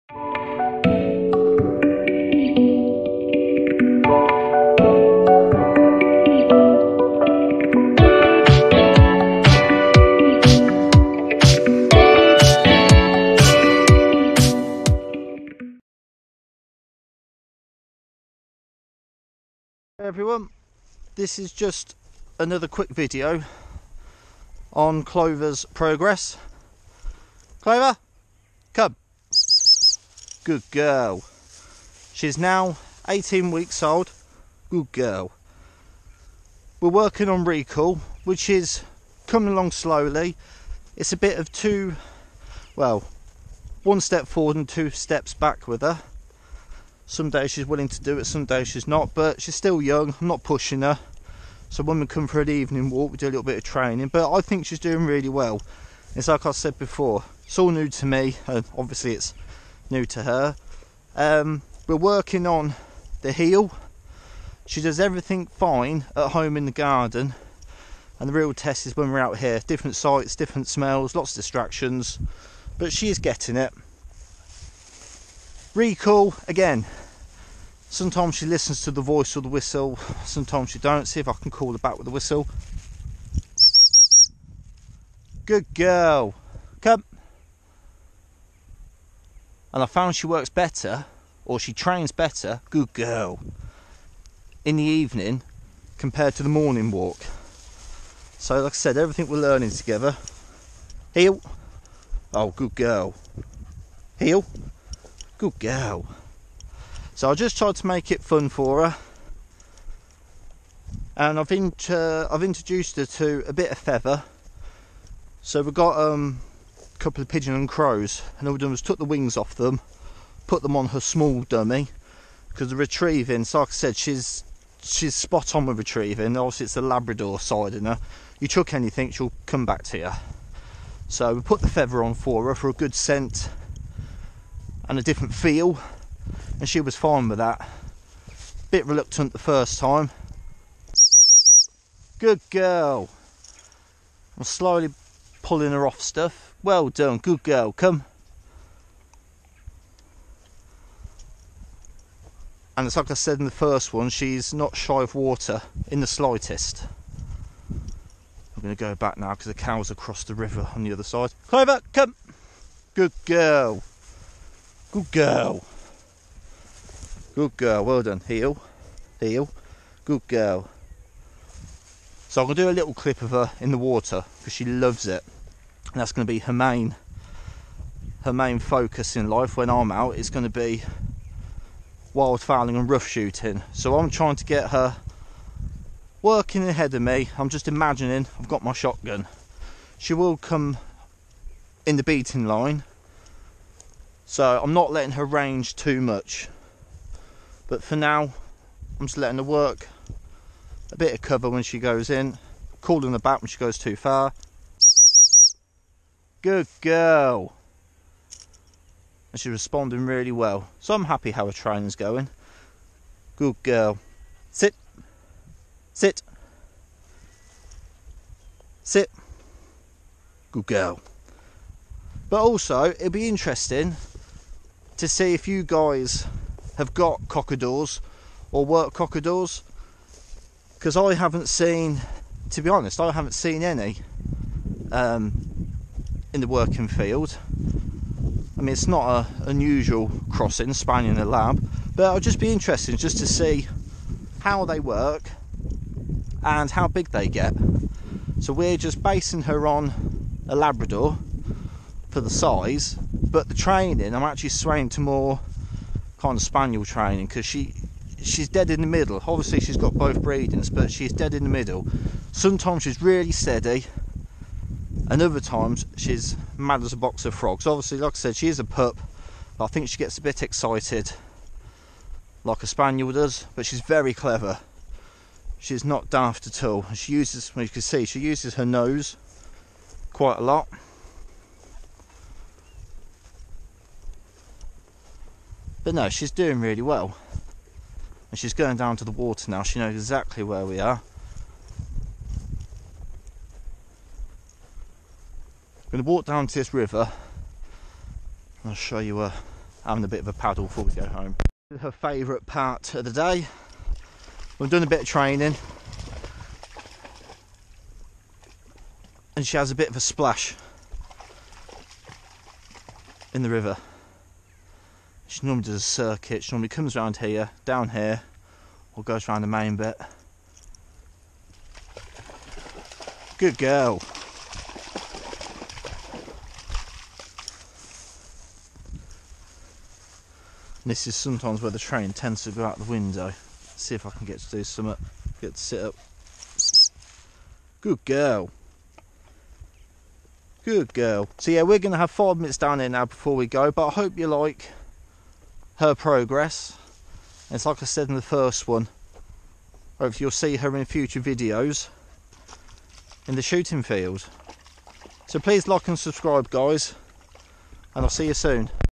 Gundog training- Clovers Progress part 2
Just a short film on how clover the cockerdoir is doing.
She is now 18 weeks old and sitting is no problem, recall is coming along nicely along with heel.